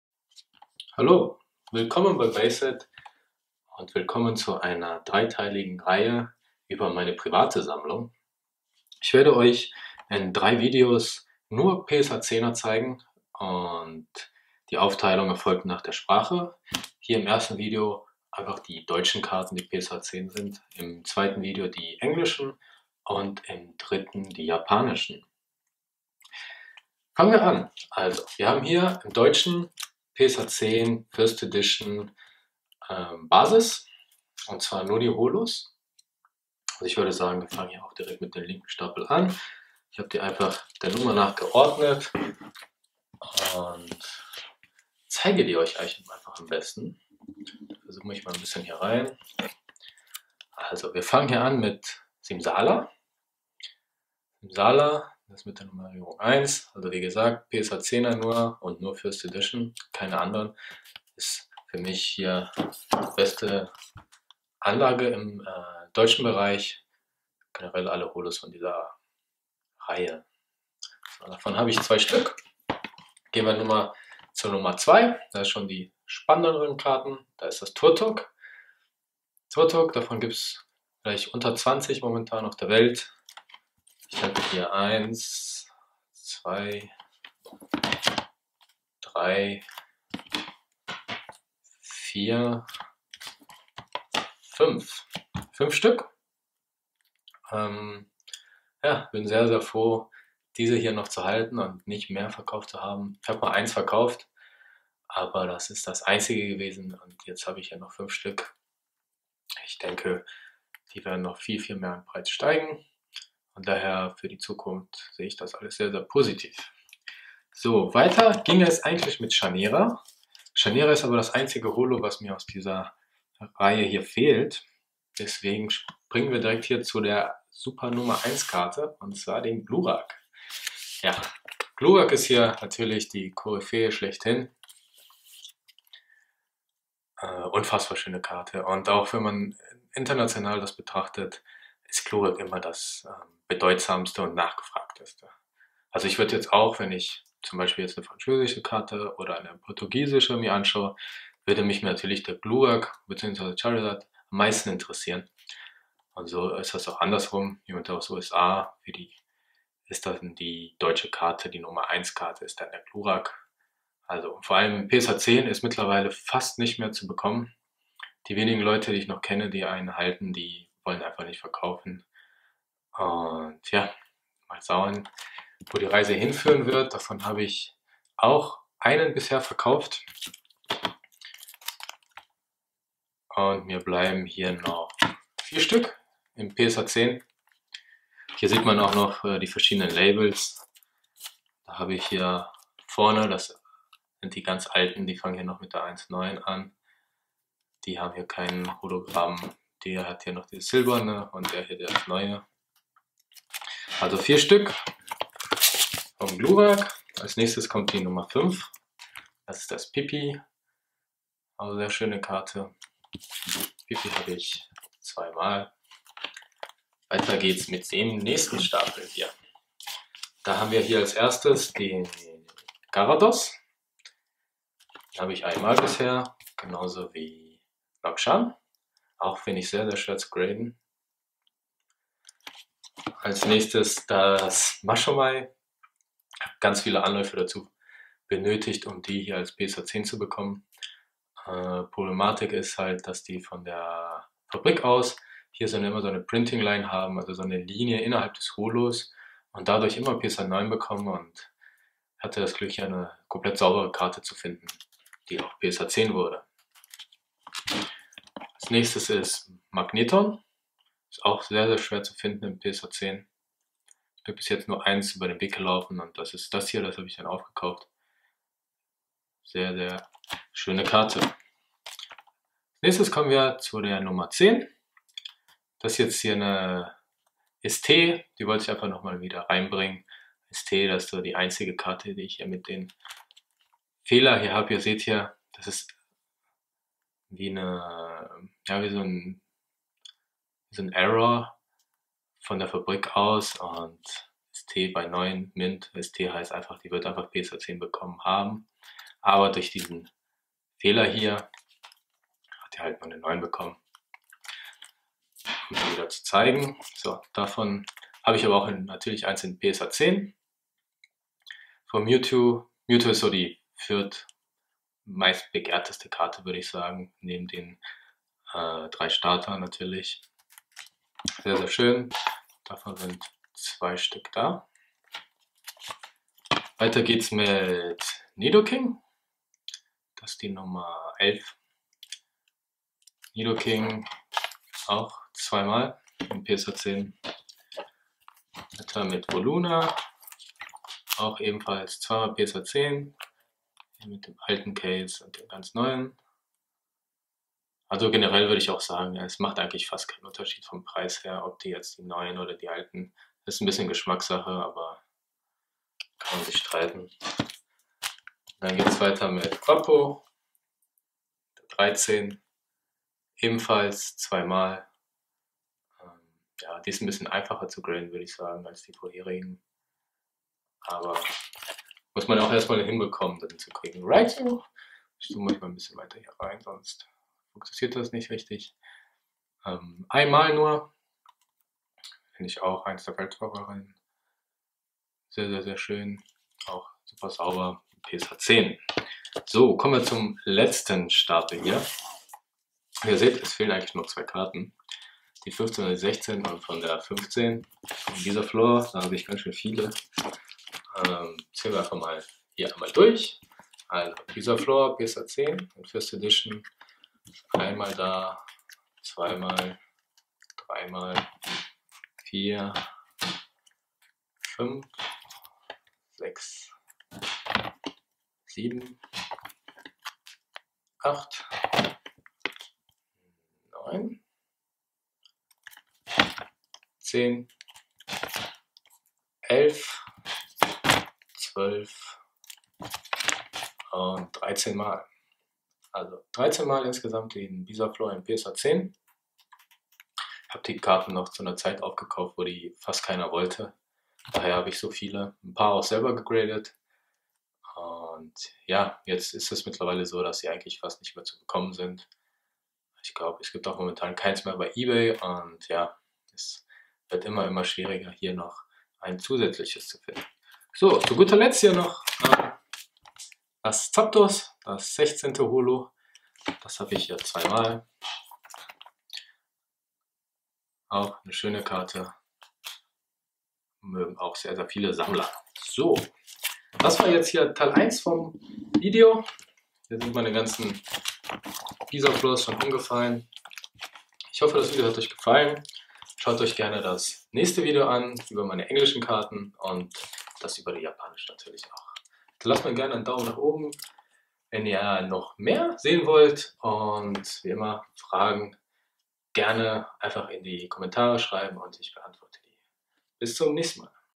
Größte Pokemon PSA10 Sammlung Deutschlands 2018 - PSA10 1st Edition Base Teil 1 (deutsch)
Willkommen zum ersten Teil meiner privaten Pokemon PSA10 Sammlung. Zum Anfang zeige ich euch meine deutschen PSA10 1st Edition Holos. Es ist die weltweit größte Sammlung an deutschen PSA10ern. Viel Spaß :)